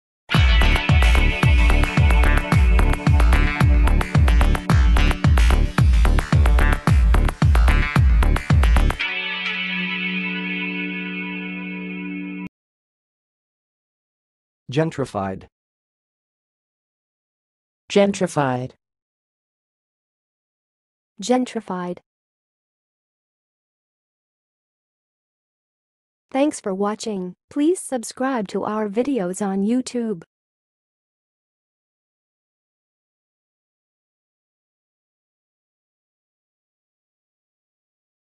How to say "gentrified"! (High Quality Voices)
Watch in this video how to say and pronounce "gentrified"!